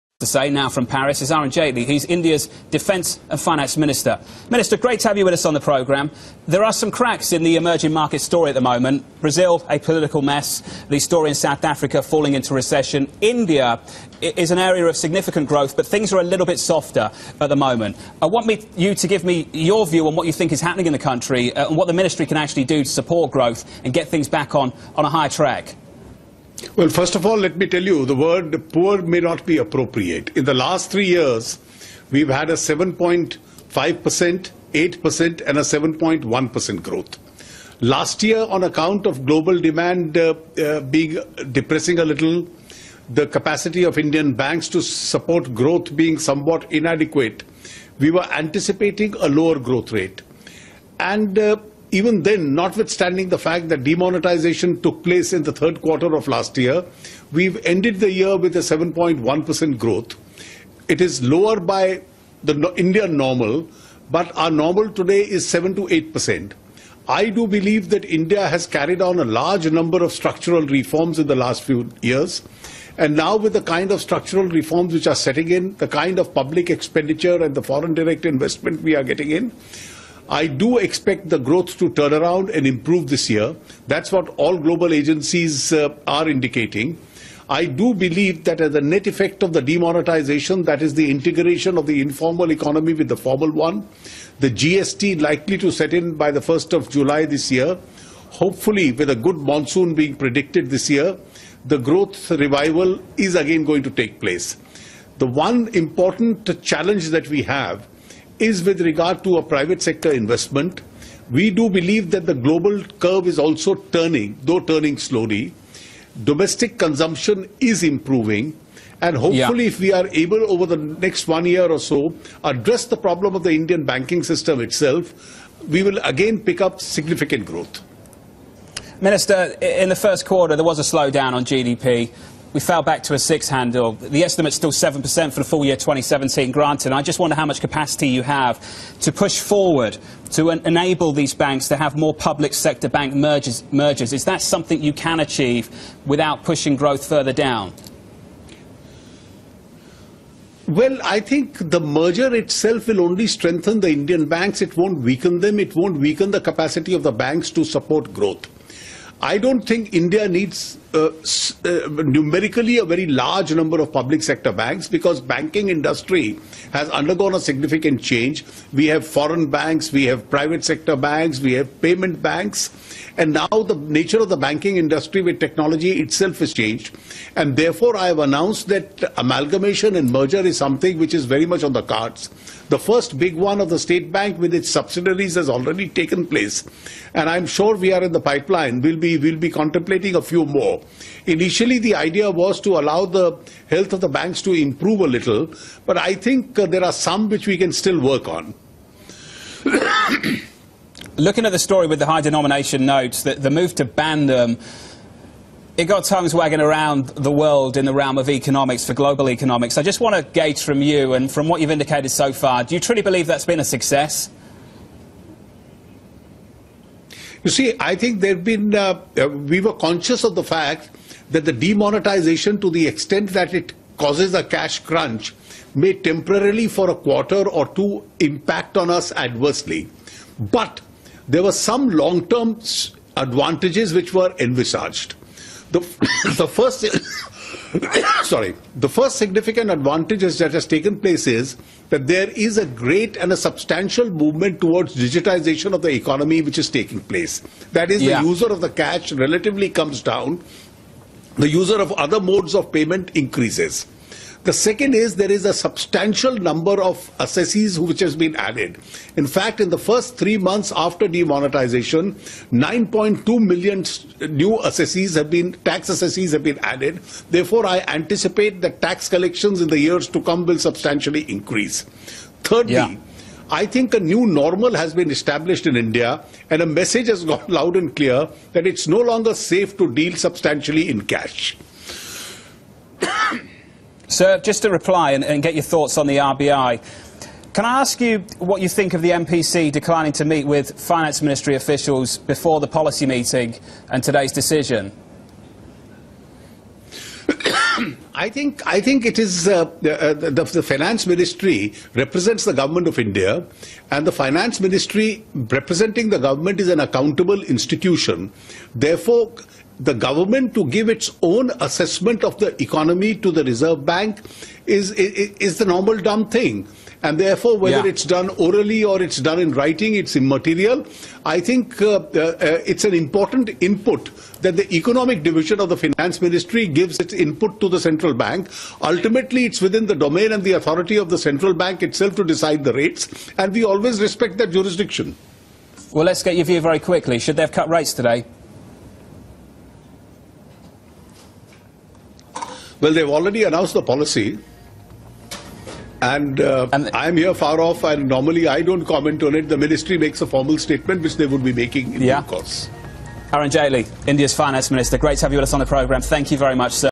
Arun Jaitley Says 'Respect' RBI's Rate Jurisdiction
First of all let me tell you the word poor may not be appropriate. In the last 3 years we have had a 7.5%, 8% and a 7.1% growth. Last year on account of global demand being a little depressed, the capacity of Indian banks to support growth being somewhat inadequate, we were anticipating a lower growth rate. And even then, notwithstanding the fact that demonetisation took place in the third quarter of last year, we have ended the year with 7.1% growth. Our normal today is 7-8%.